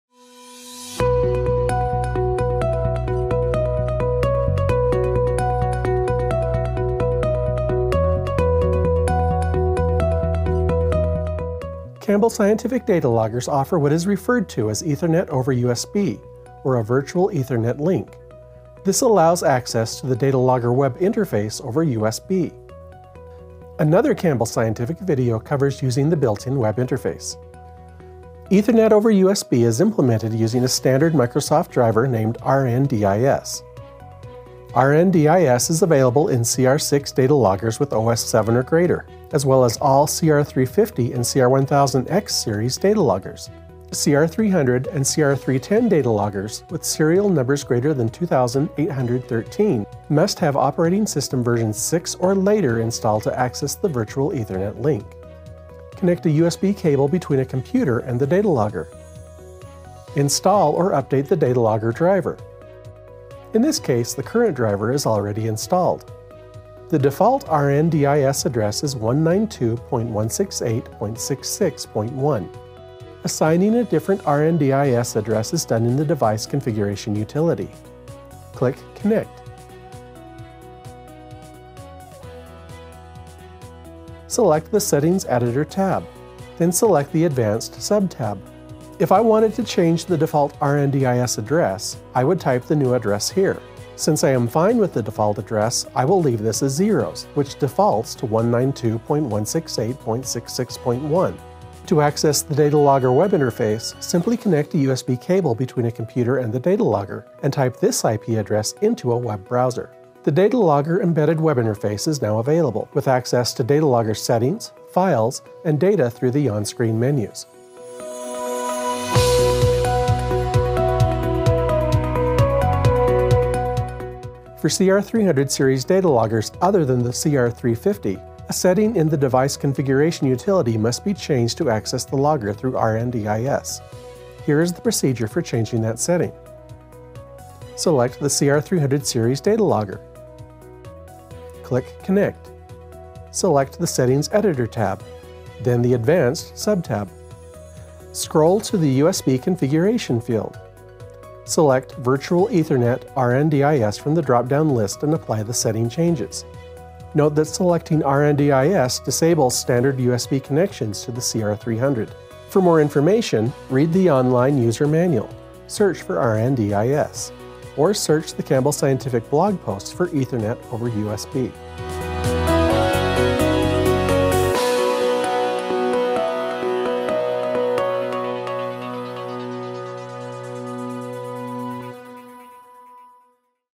Ethernet over USB
After installing Microsoft’s Remote Network Driver Interface Specification (RNDIS) driver, you can connect to your CR6, CR1000X-series, CR350-series, CR300-series, and GRANITE dataloggers via a virtual Ethernet link over USB. This connection provides access to the data loggers’ built-in web interfaces, where you can view real-time and stored data, check status and settings, manage files, and more. For details on using the web interface, watch our instructional video